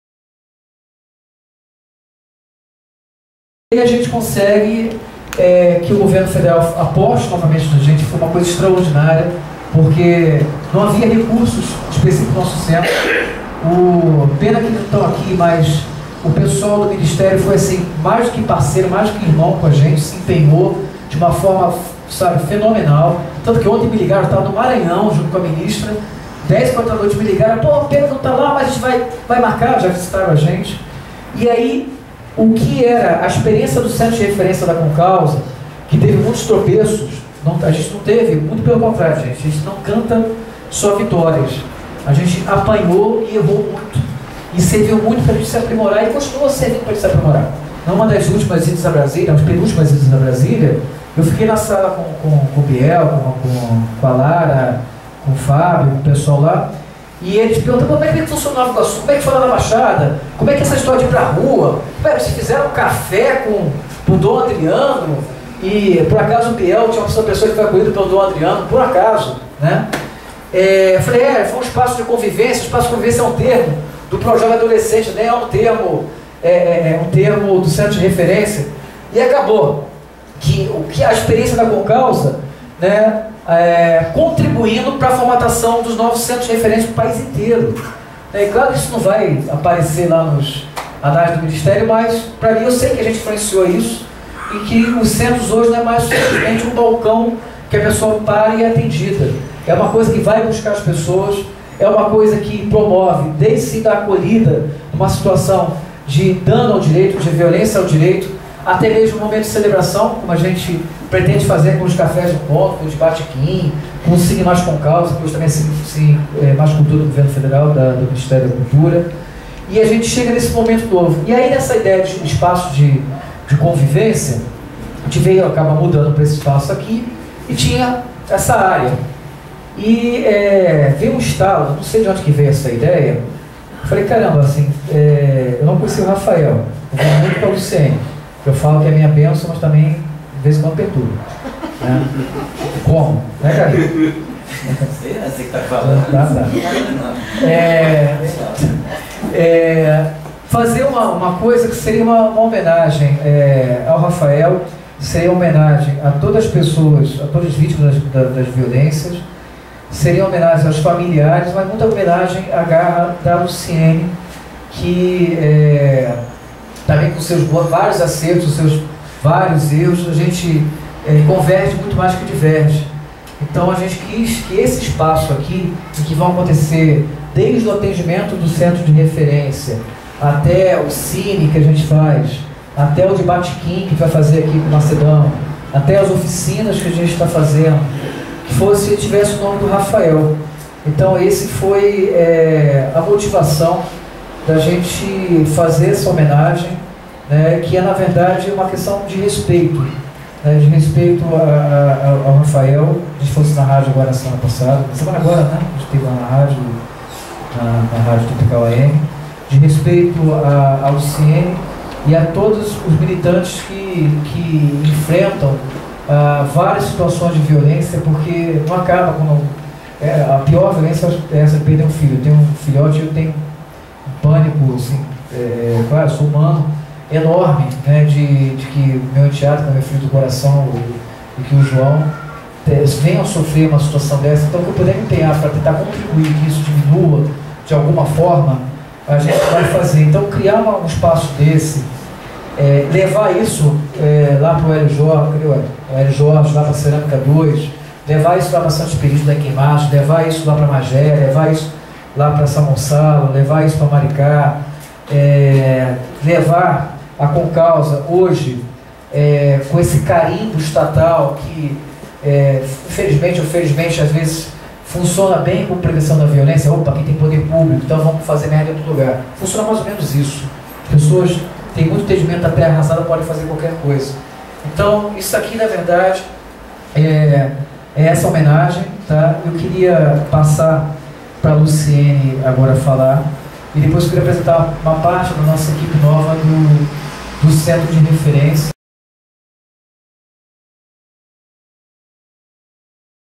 Lançamento EC CRDH Raphael Silva - Parte 3
Coordenador da ComCausa, Adriano Dias, fala sobre o Centro de Referência em Direitos Humanos.

O Café no Ponto de inauguração do Espaço de Convivência Raphael Silva Couto - Centro de Referência em Direitos Humanos (CRDH), aconteceu no último dia 31 de agosto. O evento contou com a participação de representantes comunitários, movimentos sociais, prefeituras da Baixada Fluminense e das Polícias.

| Comunicando ComCausa